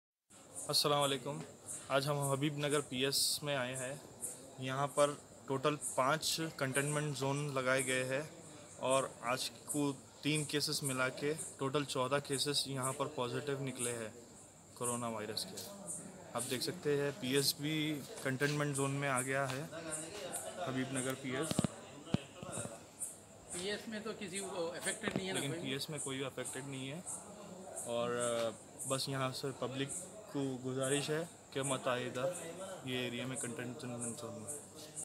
3new covid19 cases in habeeb nagar ps limits..
3new covid19 cases in habeeb nagar ps limits..Total cases reaches 14 in habeeb nagar ps limits..